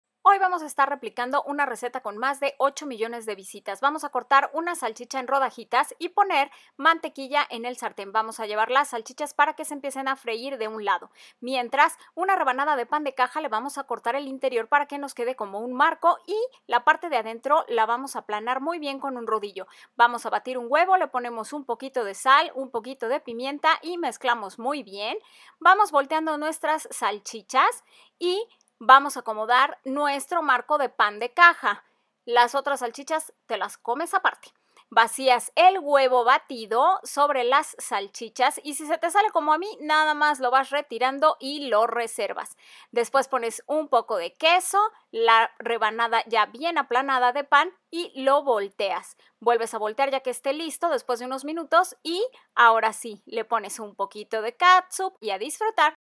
RECETA MILLONARIA, un Desayuno que te encantará
1 Salchicha
1 Rebanada de pan de caja
1 Huevo
1 loncha de Queso
Mantequilla c/n
Sal y pimienta al gusto
Salsa Catsup (opcional)